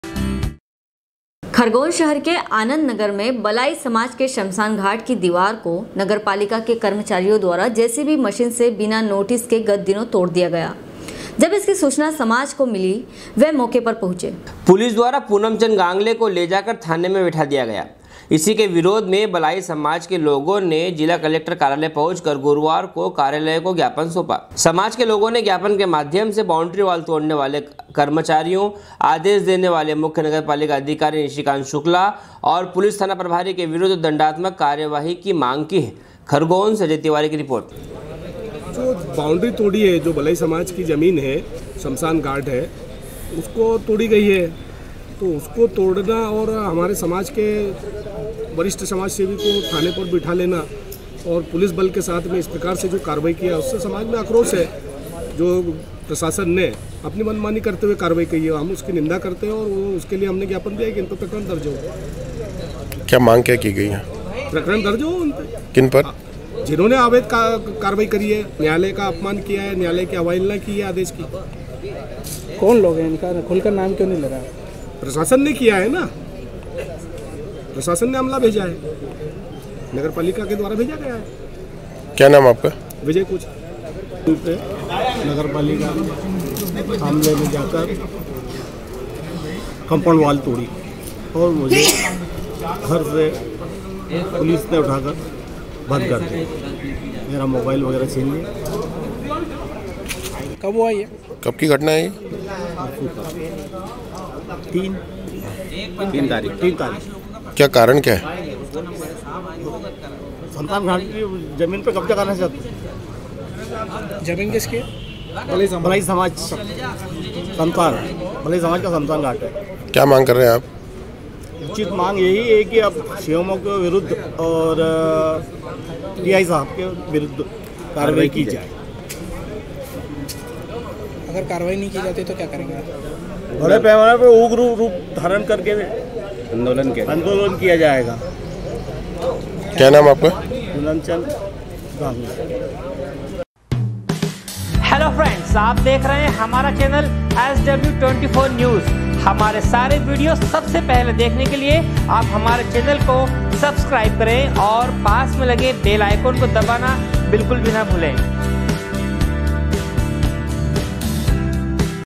sw24news khargone, 07.03.2020
खरगोन शहर के आंनद नगर मे बलाई समाज के शमशानघाट की दिवार को नगर पालिका के कर्मचारियों द्वारा जेसीबी मशीन से बिना नोटीस के गत दिनो तोड दिया गया। जब इसकी सूचना समाज को मिली ओर मौके पर पहुंचे खरगोन से अजय तिवारी की रिपोर्ट#sw24news